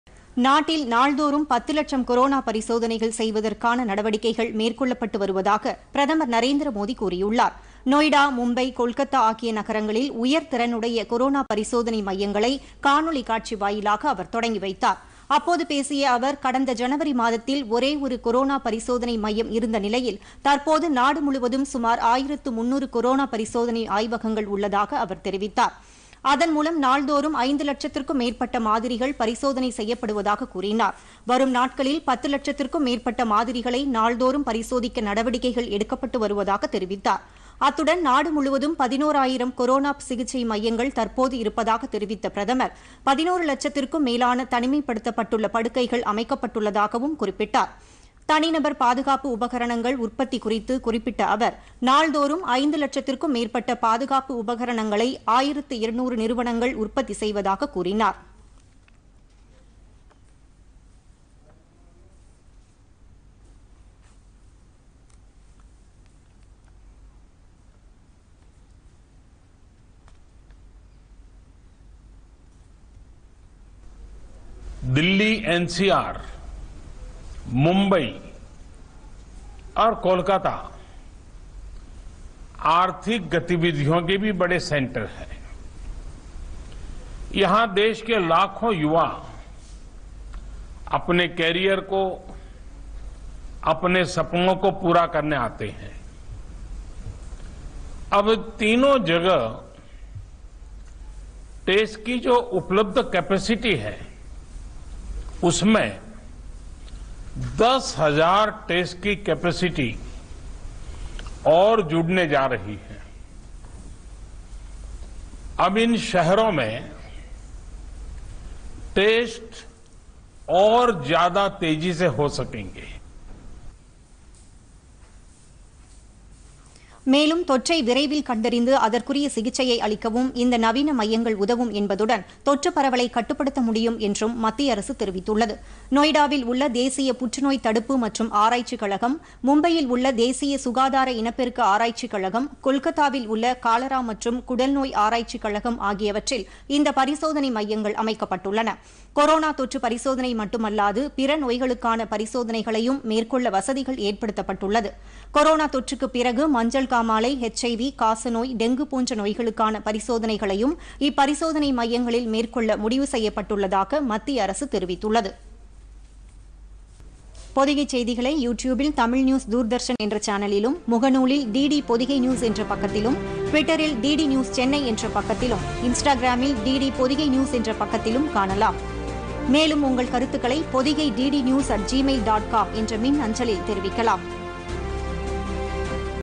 3 இடங்களில் உயர்திறன் பரிசோதனை மையங்கள்
நொய்டா, கொல்கத்தா, மும்பையில் அமைக்கப்பட்டுள்ள உயர் திறனுடைய கொரோனா பரிசோதனை  மையங்களை  காணொலி காட்சி மூலம் தொடங்கி வைத்தார் பிரதமர் நரேந்திரமோதி.